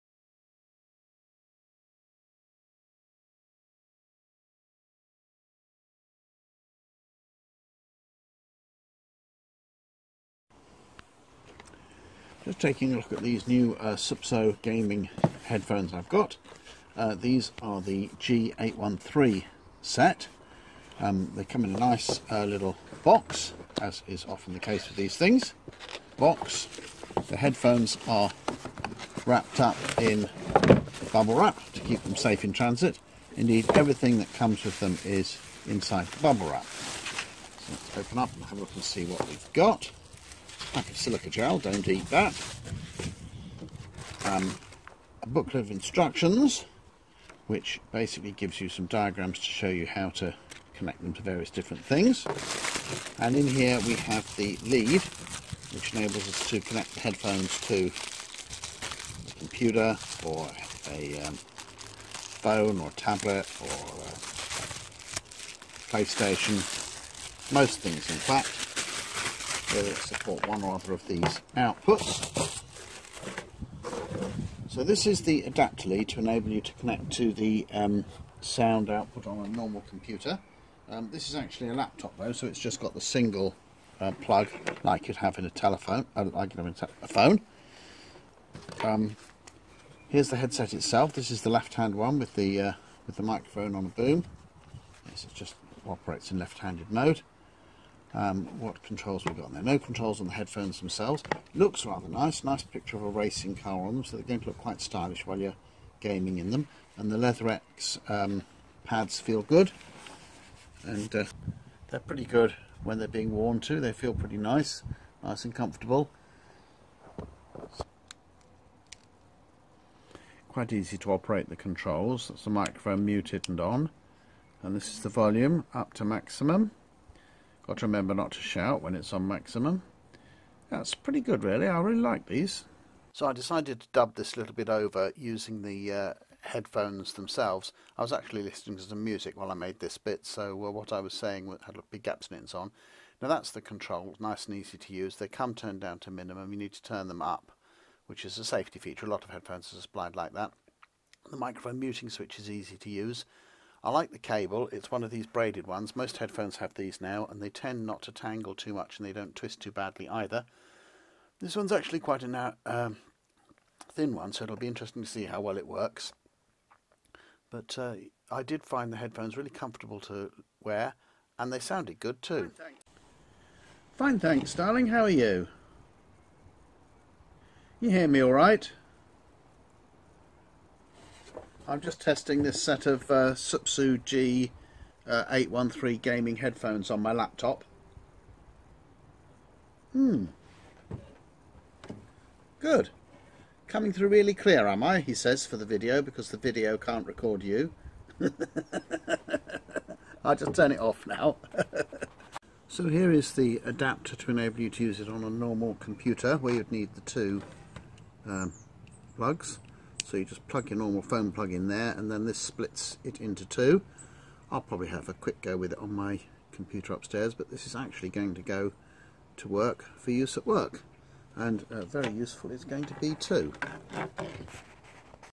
Supsoo G813
A really good set of basic gaming headphones for under £24.  There is a lot to like about these headphones form the light weight to the comfort ease of adjustment and really good sound.  They do mute ambient sound quite well and should serve you well for almost any use you might want a headset for.  You can of course get better quality but at this price point these are really good.